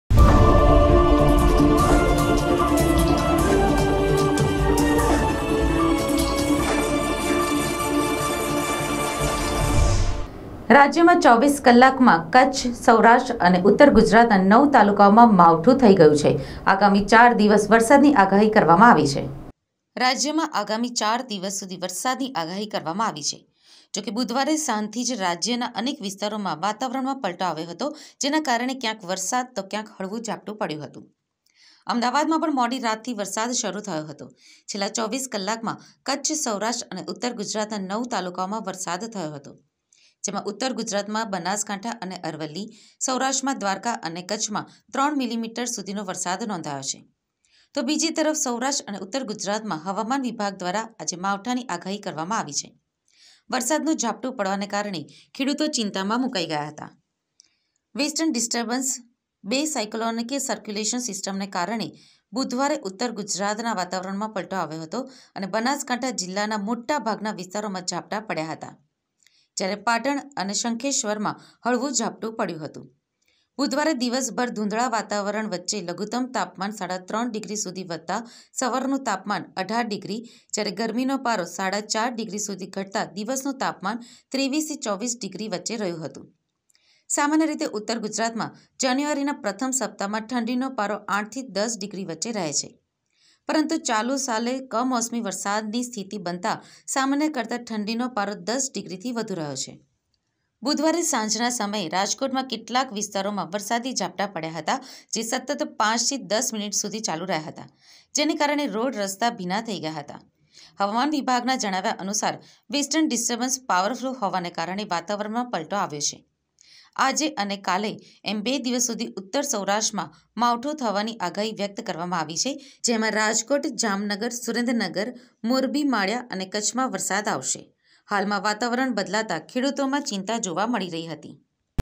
ઉ.ગુજરાતના વાતાવરણમાં પલટો આવતાં અનેક વિસ્તારોમાં વરસાદ થયો
રાજ્યમાં 24 કલાકમાં કચ્છ, સૌરાષ્ટ્ર અને ઉ.ગુજરાતના 9 તાલુકાઓમાં માવઠું, આગામી ચાર દિવસ વરસાદની આગાહી